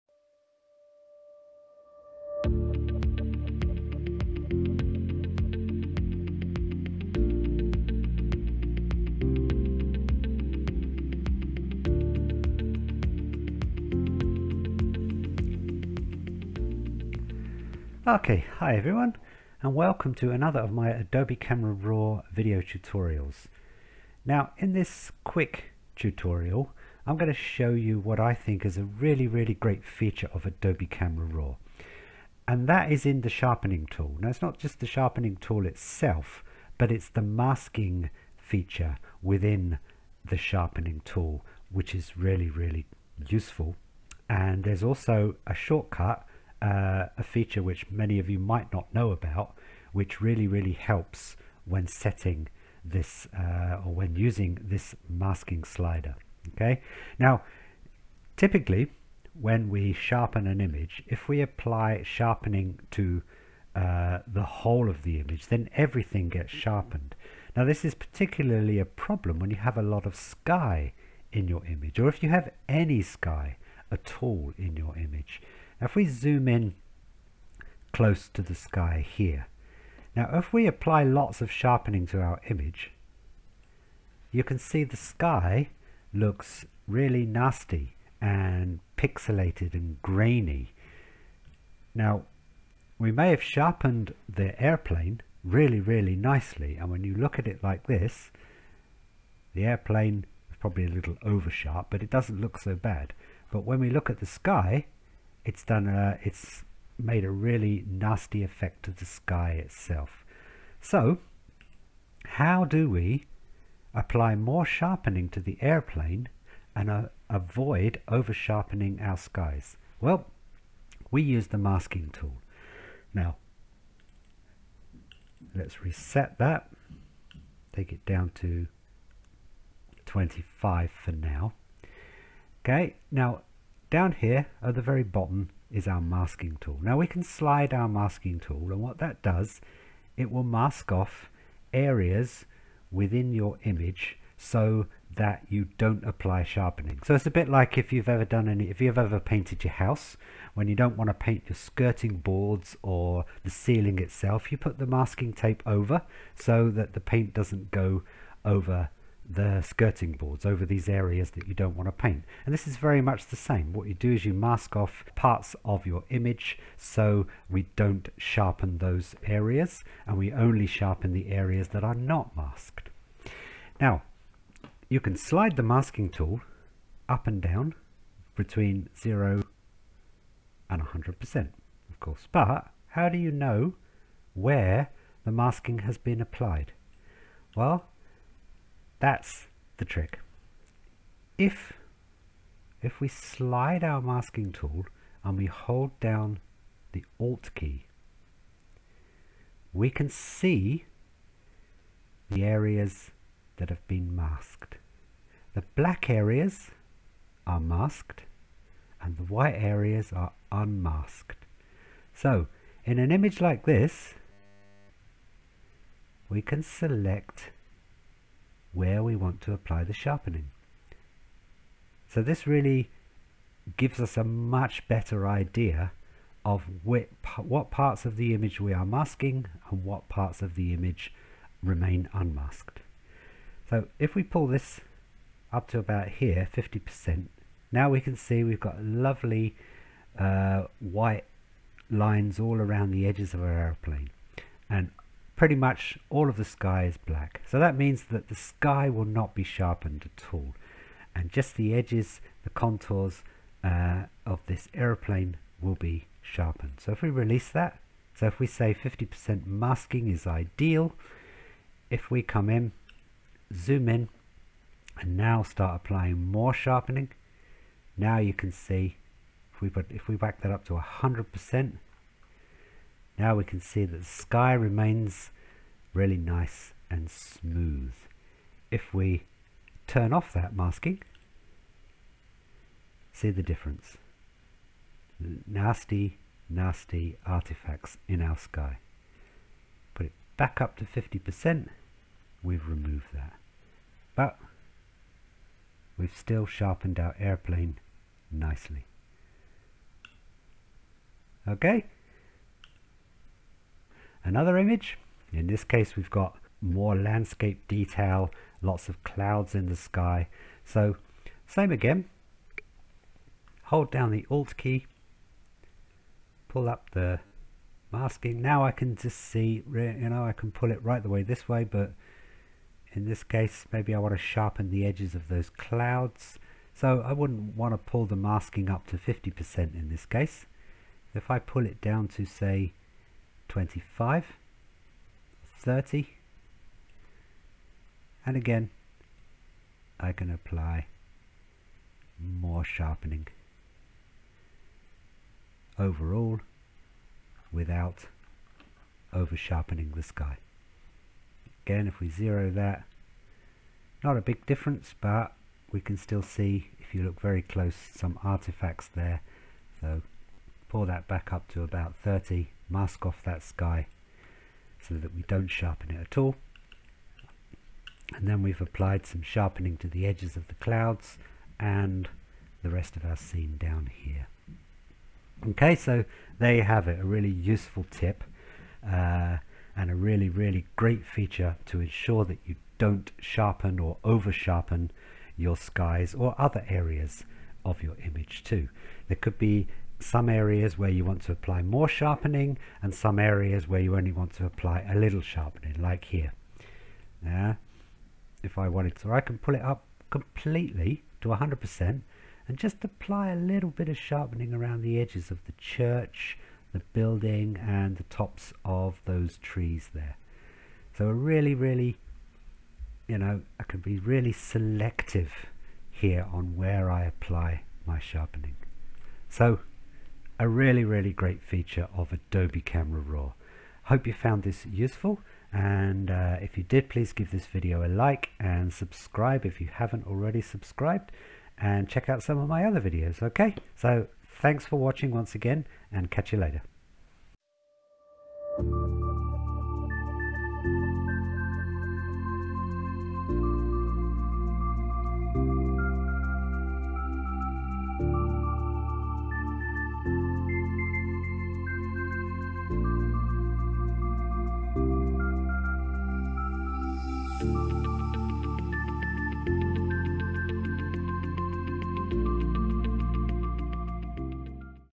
How to avoid sharpening your skies with the sharpening mask tool in Adobe Camera RAW.
In this quick photography editing tutorial, I show you a handy tip on how to effectively use the sharpening mask tool to avoid sharpening your skies and also oversharpening other elements in your photo using Adobe Camera RAW in Photoshop.

The problem with overall sharpening is that when it comes to skies, it can actually ruin and make them look pixelated. In this video tutorial I show you how to easily sharpen the main features of your image without affecting the sky by using the masking tool in Adobe Camera RAW along with a shortcut to help you see where you are applying the masking.

All my videos, tutorials and articles are provided free of charge.